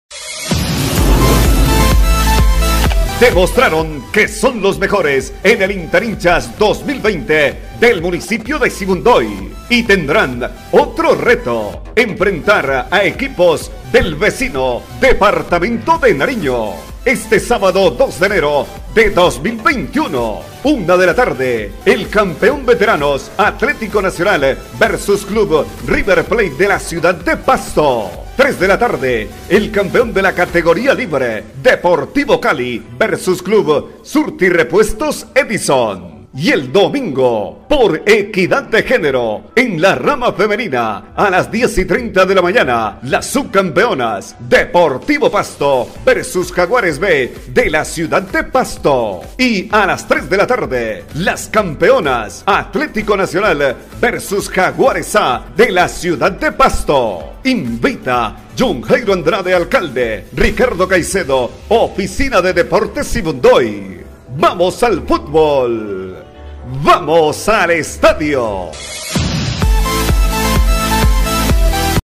Deporte Fin de Semana 2021 Enero 2 y 3
La Administración Municipal, Jhon Jairo Andrade Caicedo (Alcalde), Janeth Cristina Cárdenas (Gestora Social), y la Coordinación de Deportes invitan a disfrutar del gran evento futbolero este fin de semana 2 y 3 de enero de 2021. Disfrutemos cumpliendo con el distanciamiento social requerido y aplicando las normas de bioseguridad exigidas...